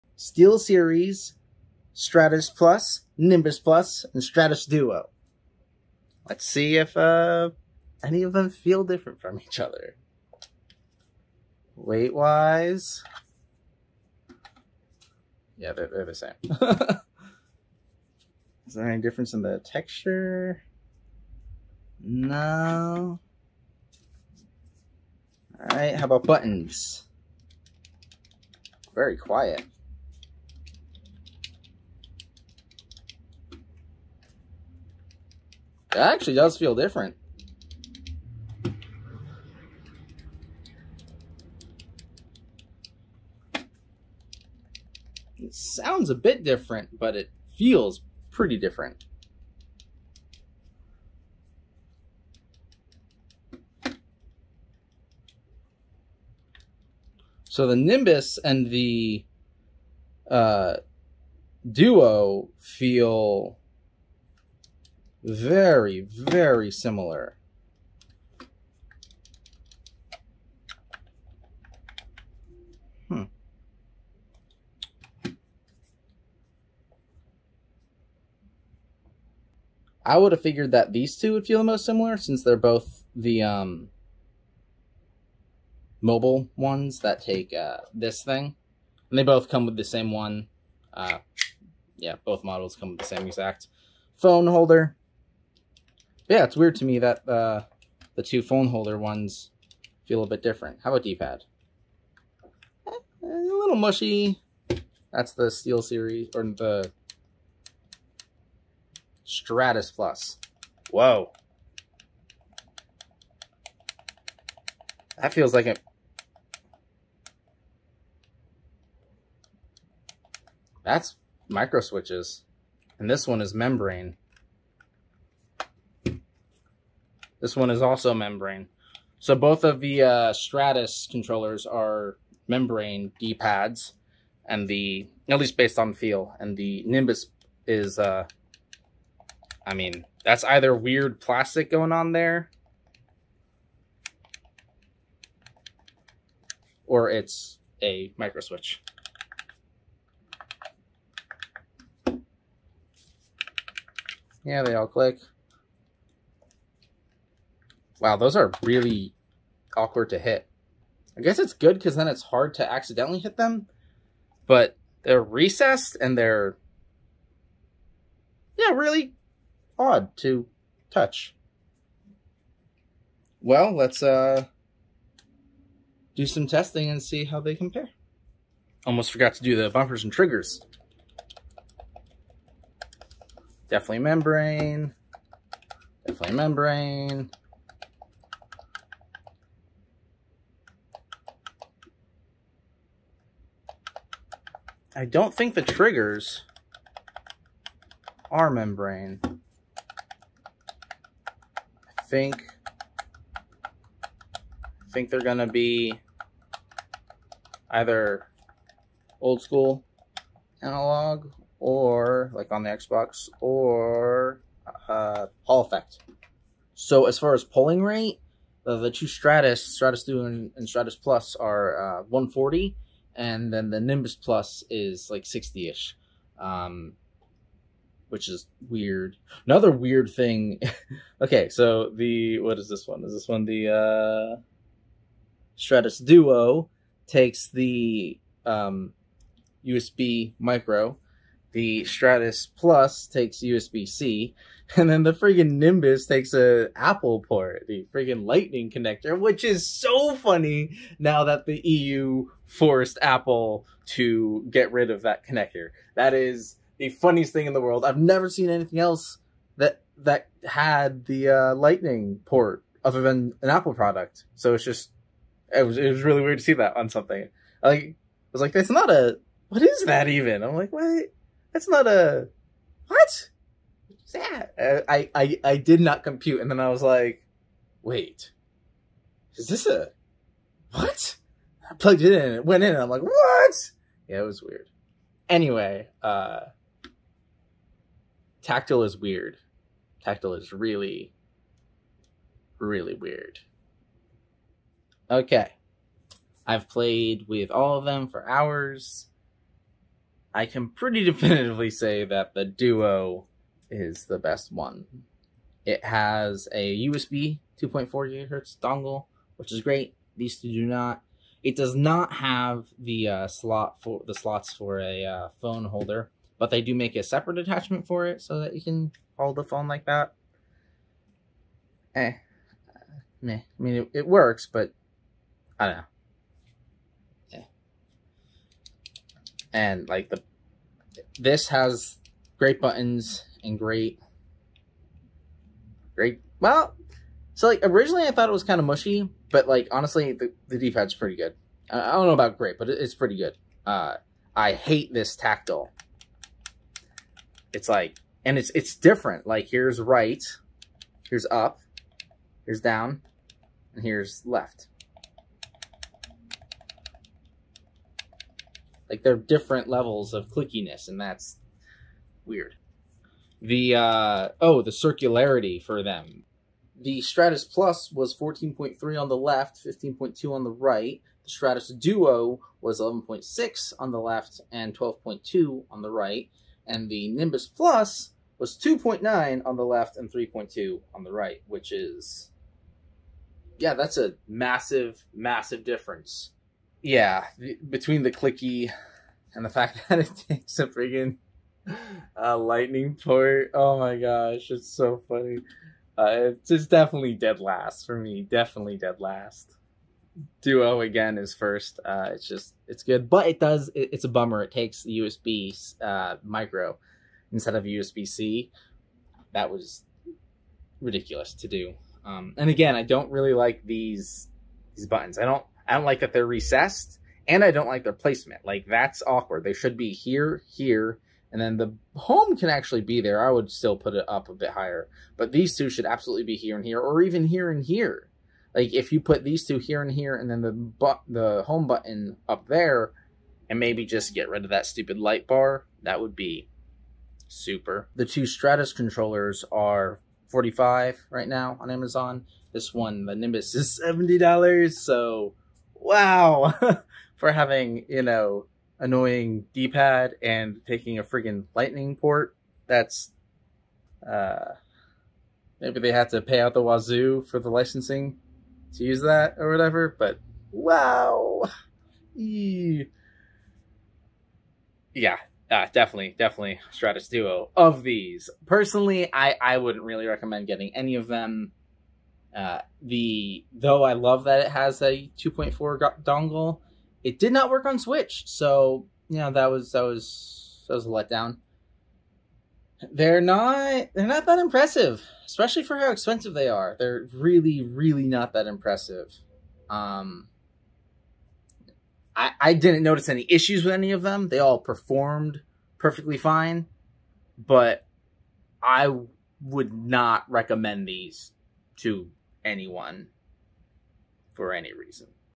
Steel Series Stratus+, Stratus Duo, Nimbus+ - Overpriced
Stratus Duo

00:00 Beginning
00:11 Weight Differences?
00:26 Face Buttons
01:41 D-Pads
03:01 Bumpers and Triggers
03:37 Polling Rate
03:55 Different USB Connections
04:56 After Playing for Hours
06:27 Rankings
07:18 Worth the Money?